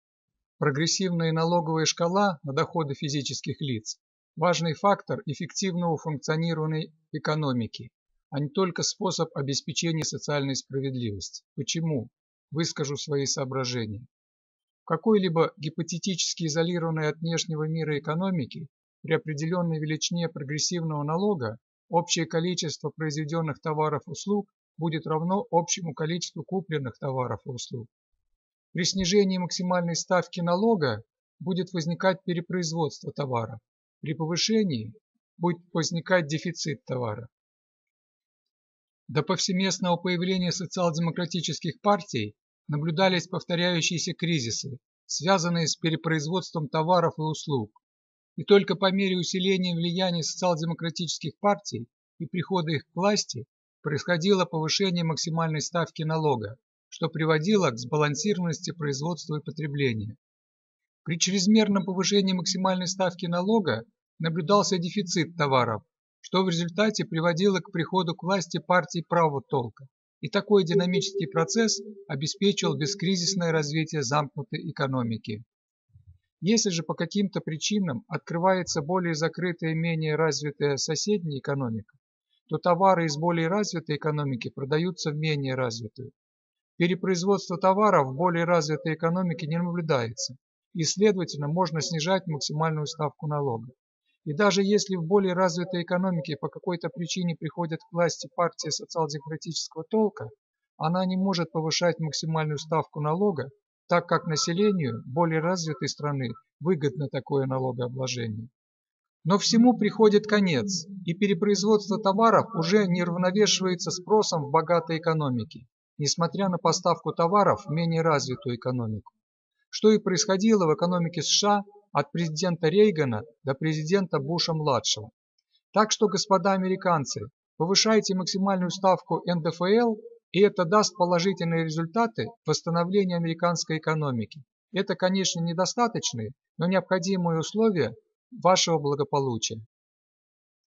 Прогрессивная шкала НДФЛ необходимое условие эффективного функционирования экономики
Прогрессивная налоговая шкала на доходы физических лиц – важный фактор эффективного функционирования экономики, а не только способ обеспечения социальной справедливости.
Почему? Выскажу свои соображения.
В какой-либо гипотетически изолированной от внешнего мира экономике при определённой величине прогрессивного налога общее количество произведенных товаров и услуг будет равно общему количеству купленных товаров и услуг. При снижении максимальной ставки налога будет возникать перепроизводство товаров, при повышении – будет возникать дефицит товаров.
До повсеместного появления социал-демократических партий наблюдались повторяющиеся кризисы, связанные с перепроизводством товаров и услуг. И только по мере усиления влияния социал-демократических партий и прихода их к власти происходило повышение максимальной ставки налога, что приводило к сбалансированности производства и потребления. При чрезмерном повышении максимальной ставки налога наблюдался дефицит товаров, что в результате приводило к приходу к власти партий правого толка. И такой динамический процесс обеспечивал бескризисное развитие замкнутой экономики.
Если же по каким-то причинам открывается ранее закрытая менее развитая соседняя экономика, то товары из более развитой экономики продаются в менее развитую. Перепроизводства товаров в более развитой экономике не наблюдается и, следовательно, можно снижать максимальную ставку налога. И даже если в более развитой экономике по какой-то причине приходит к власти партия социал-демократического толка, она не может повышать максимальную ставку налога, так как населению более развитой страны выгодно такое налогообложение.
Но всему приходит конец. И перепроизводство товаров уже не уравновешивается спросом в богатой экономике, несмотря на поставку товаров в менее развитую экономику.
Что и происходило в экономике США от президента Рейгана до президента Буша-младшего.
Так что господа американцы, повышайте максимальную ставку НДФЛ и это даст положительные результаты в восстановлении американской экономики. Это, конечно, не достаточное, но необходимое условие вашего благополучия.